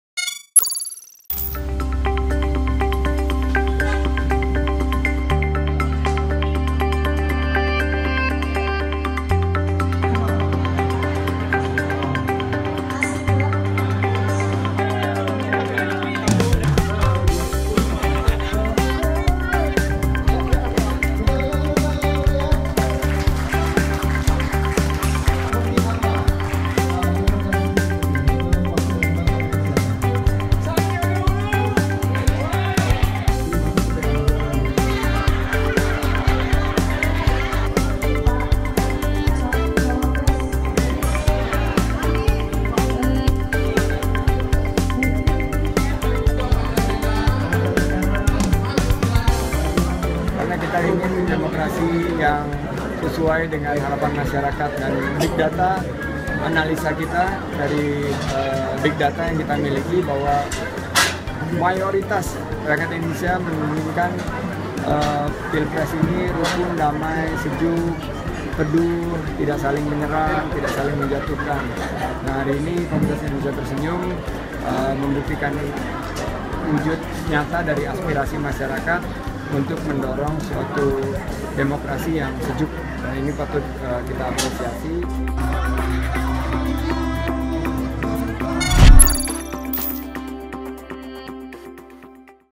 Sandi: Masyarakat Ingin Pilpres Tak Saling Serang dan Menjatuhkan
Bakal calon wakil presiden RI, Sandiaga Uno menghadiri acara diskusi publik 2019DemokrasiSejuk. Ia mengatakan masyarakat Indonesia menginginkan pilpres 2019 berlangsung sejuk.